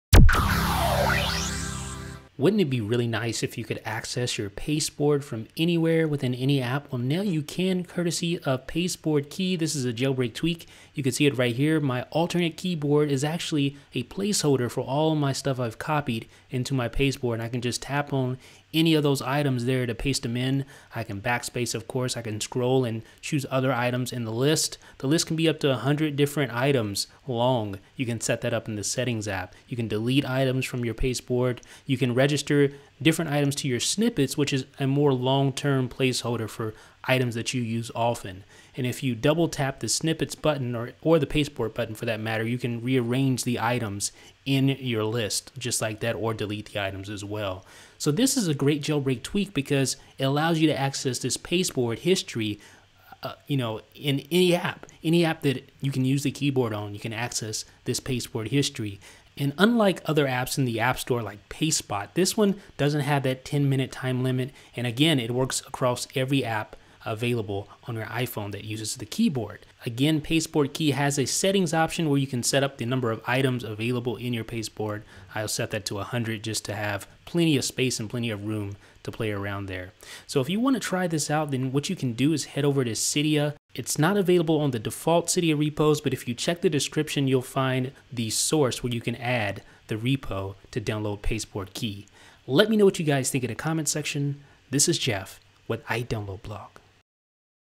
PasteboardKey
Name: PasteboardKey
Description: Access pasteboard history and snippets from any app.
Price: Free
Developer: moyashi

About iDB